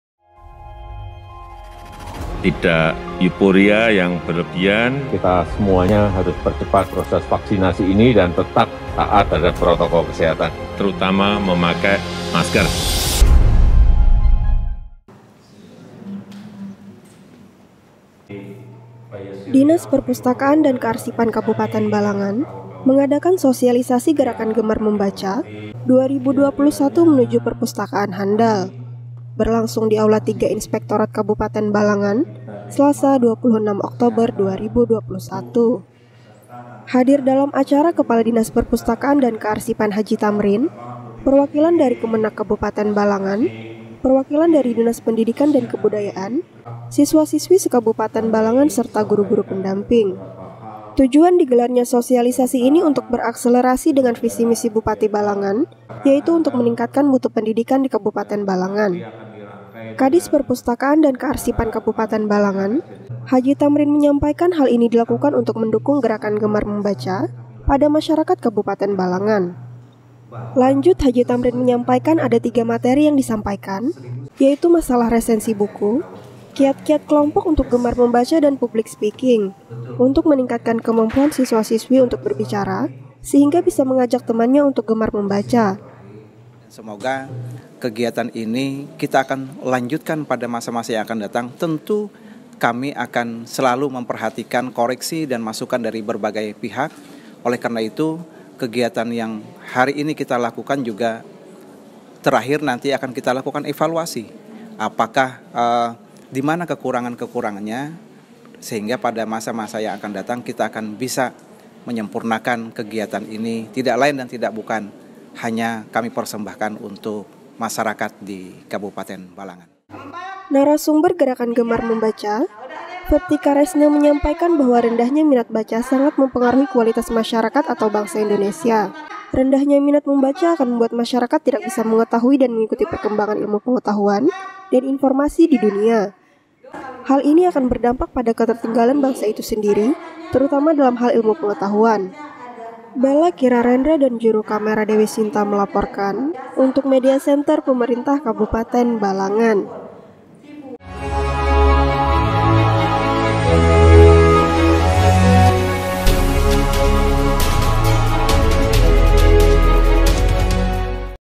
Dispersip Sosialisasikan Gerakan Gemar Membaca Guna Tingkatkan Mutu Pendidikan di Kabupaten Balangan
Dinas Perpustakaan Dan Kearsipan Kabupaten Balangan Mengadakan Sosialisasi Gerakan Gemar Membaca 2021 Menuju Perpustakaan Handal, Berlangsung di Aula 3 Inspektorat Kabupaten Balangan, Selasa (26/10/21).